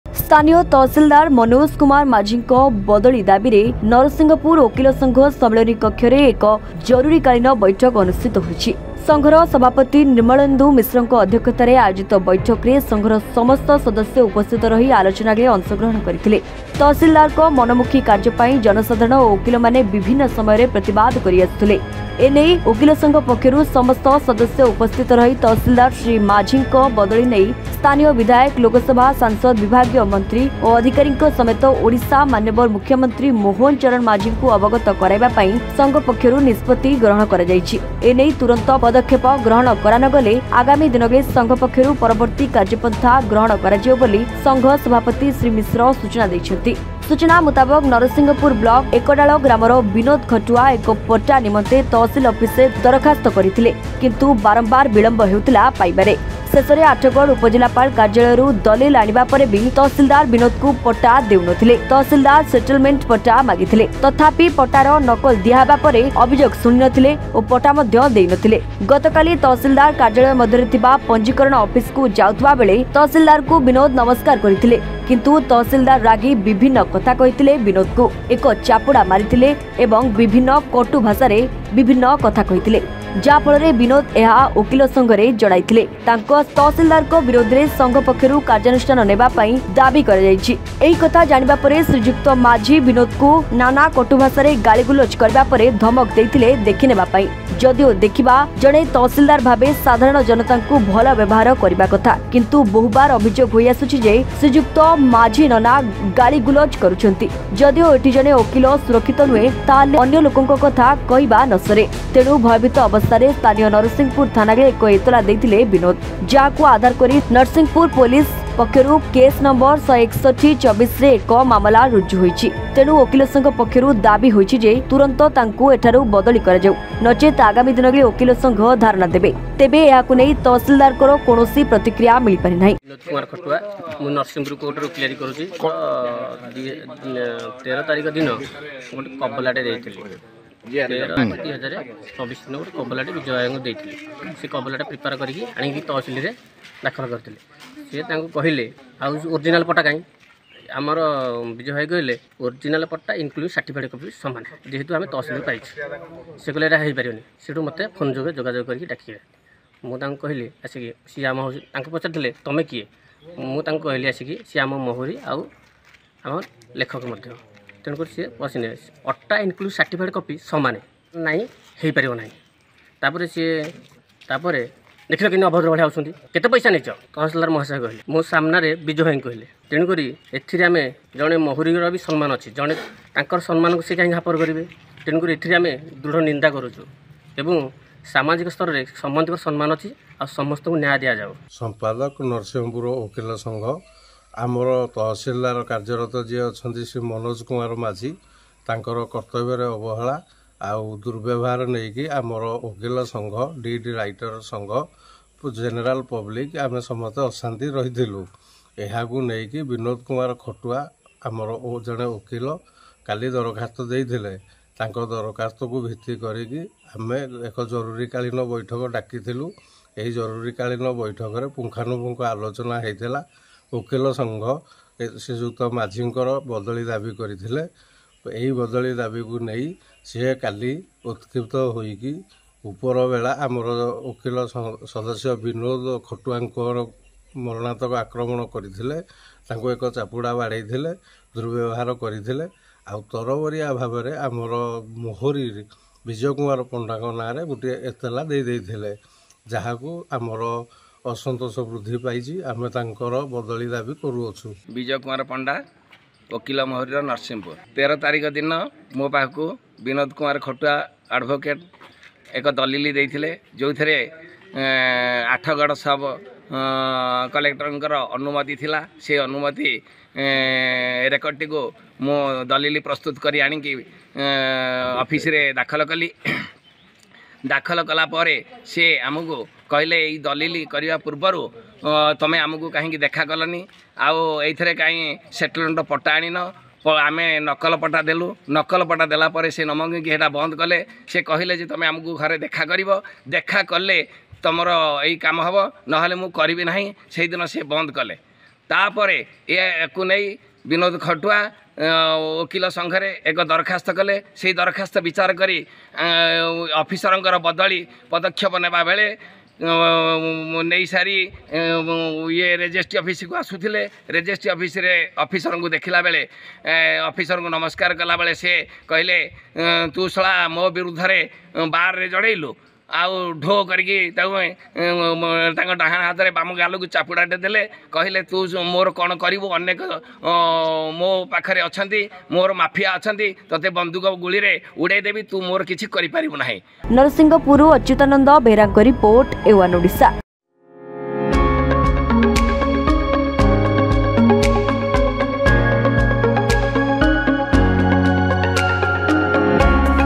ତହସିଲଦାରଙ୍କ ମନମୁଖୀ କାର୍ଯ୍ୟ ପାଇଁ ଓକିଲ ସଂଘ ସମ୍ମିଳନୀ କକ୍ଷରେ ଜରୁରୀ ବୈଠକ ଅନୁଷ୍ଠିତ |CUTTACK NEWS
ତହସିଲଦାରଙ୍କ ମନମୁଖୀ କାର୍ଯ୍ୟ ପାଇଁ ଓକିଲ ସଂଘ ସମ୍ମିଳନୀ କକ୍ଷରେ ଜରୁରୀ ବୈଠକ ଅନୁଷ୍ଠିତ  @a1odisha